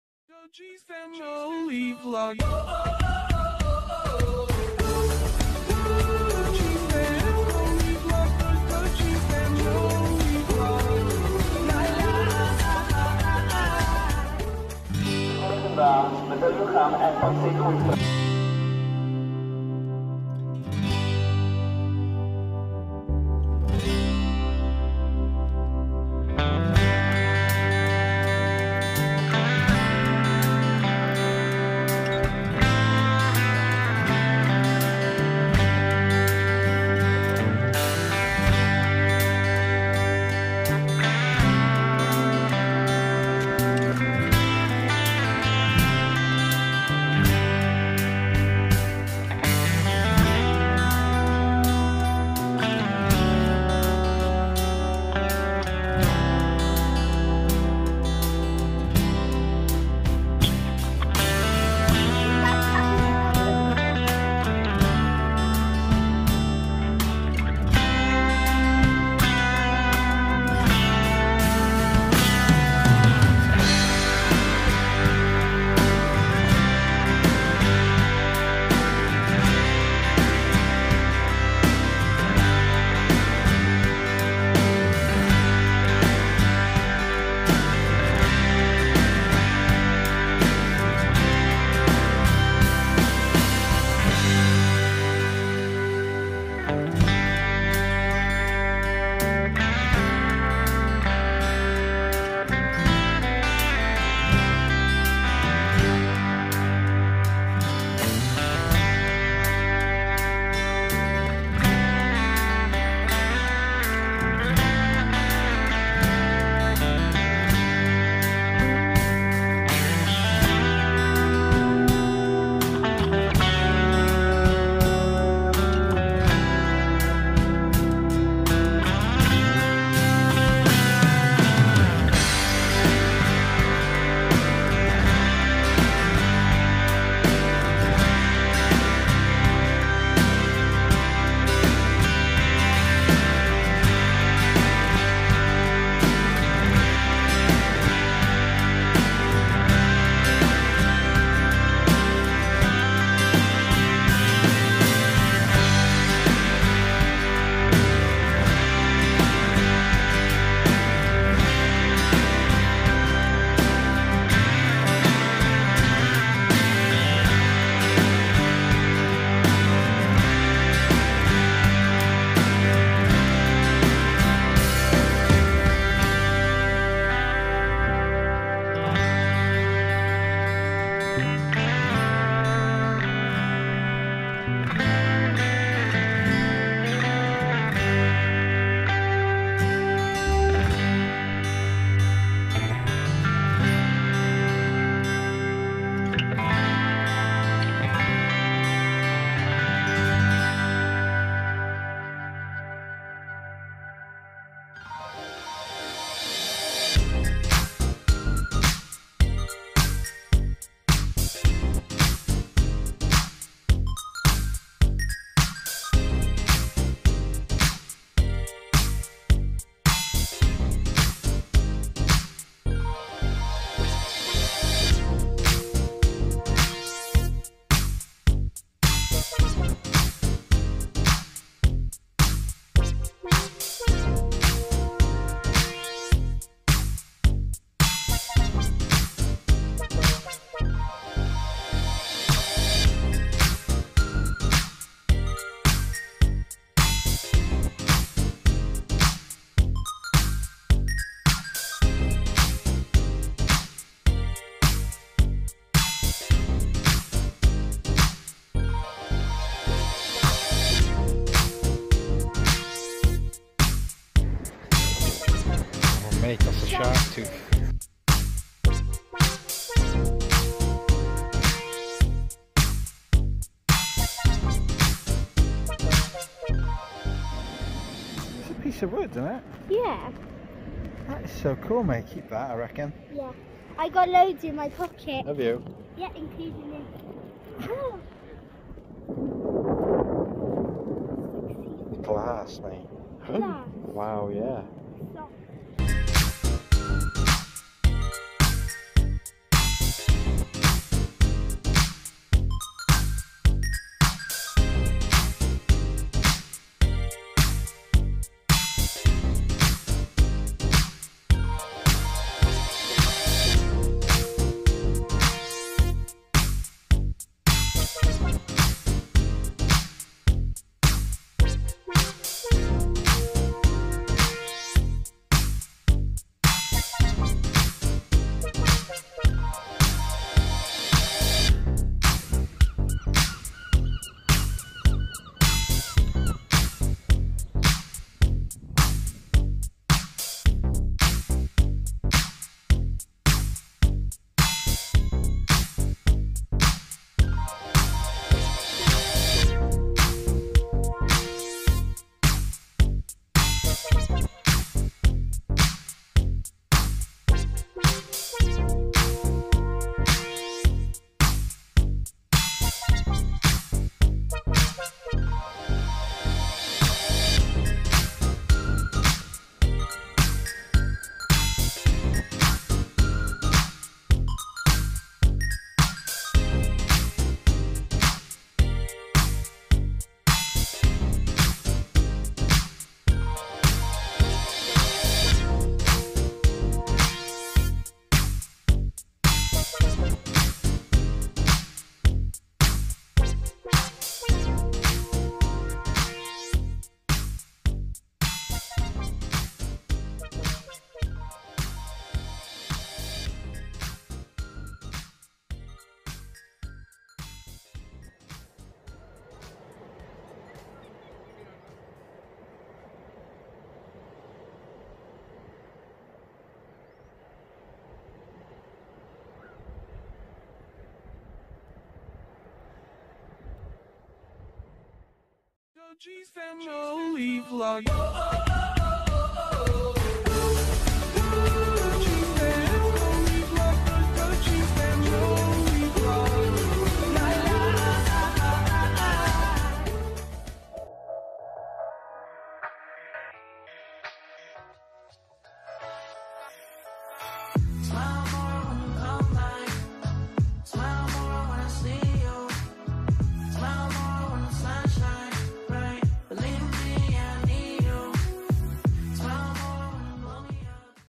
Barry Island 7 May 2020
Take A musical walk with us to a empty Barry Island 💙